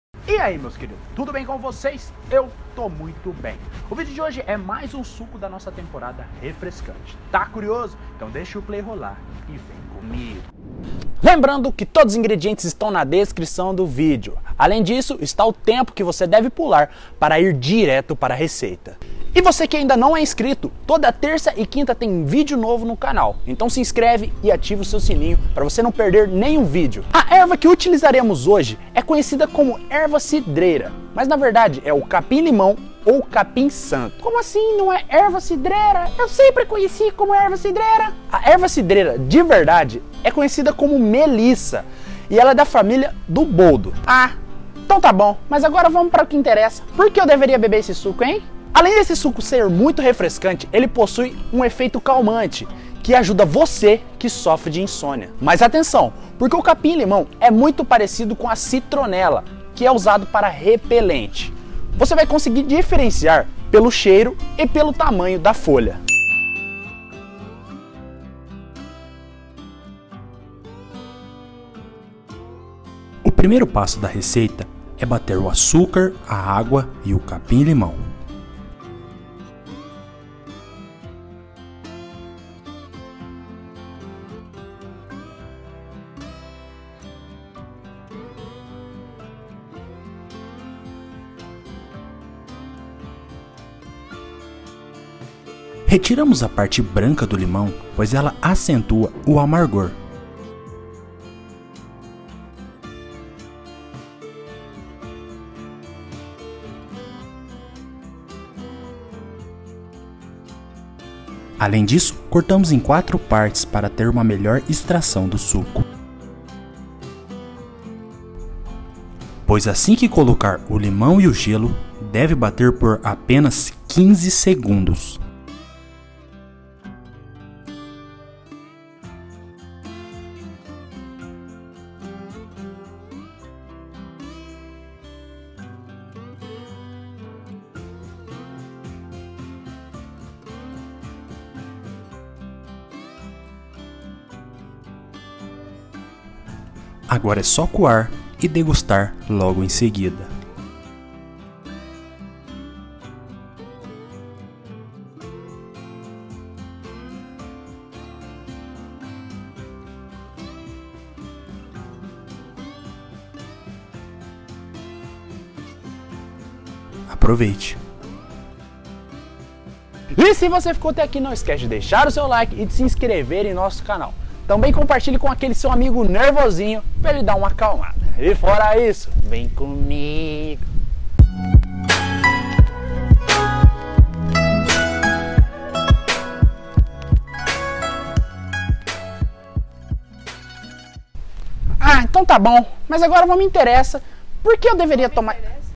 SUCO REFRESCANTE QUE ACALMA! | RECEITA FÁCIL
A receita começa em: 1:13
Ingredientes:
1 litro de água
1 limão taiti
4 folhas de capim limão
7 colheres de sopa de açúcar (150g)
16 cubos de gelo

Nível da receita: Fácil
Custo aproximado:  R$ 1,50
Serve: 4 pessoas
Este suco é muito bom para quem tem insônia, o capim limão ou capim santo ajuda você que tem problemas para dormir. Ele acalma os nervos e além disso é muito refrescante para o verão. Suco para toda família, muito barato de ser feito e muito delicioso!